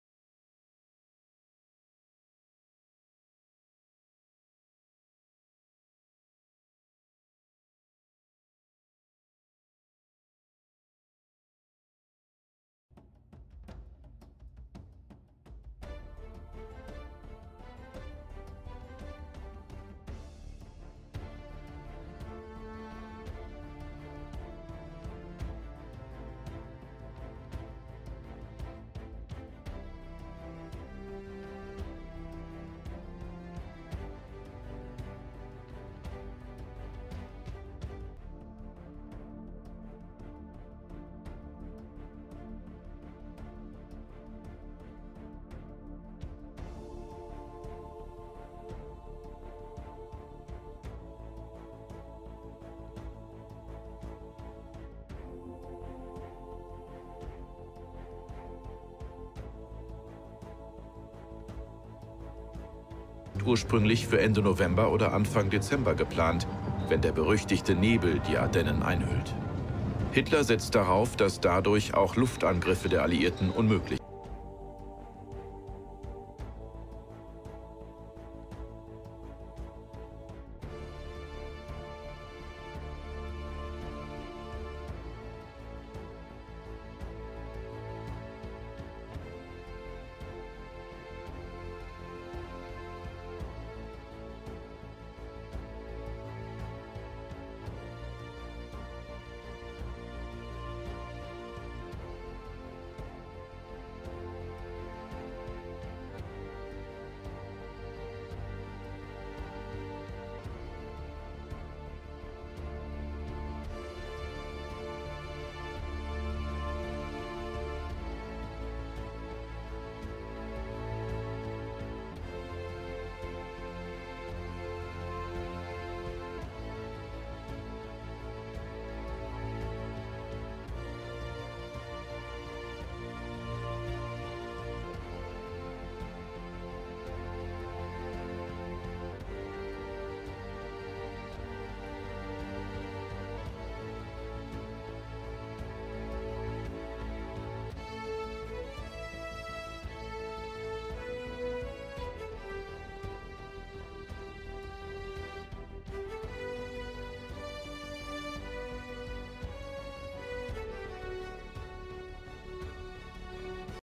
THE LONGEST MARCH
COMPILATION OF COLORIZED ORIGINAL FILMS ABOUT WEHRMACHT TROOPS MARCHING LONG RANGES DURING WW2
The video you will be watching is compiled from colorized real movies containing scenes about German infantry marching long ranges during ww2

It was prepared for historical information and educational purposes only.

it DOESN'T AIM to humiliate, disgrace, underestimate, insult, praise, glorify, blame or bless any particular nation, state, country, political regime, ideology or person/persons in any way.